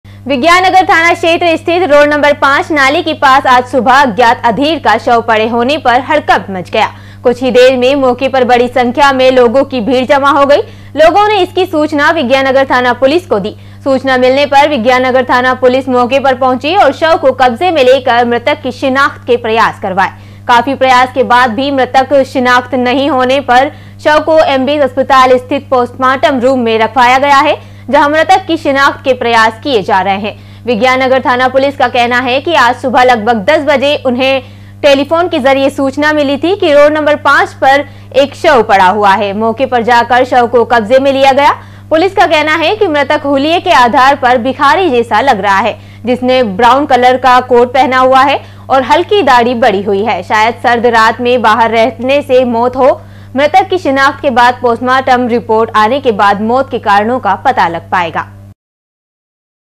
11 JAN.2022 @NEWS कोटा - अज्ञात अधेड़ का शव मिलने से मचा हड़कंप
कोटा - अज्ञात अधेड़ का शव मिलने से मचा हड़कंप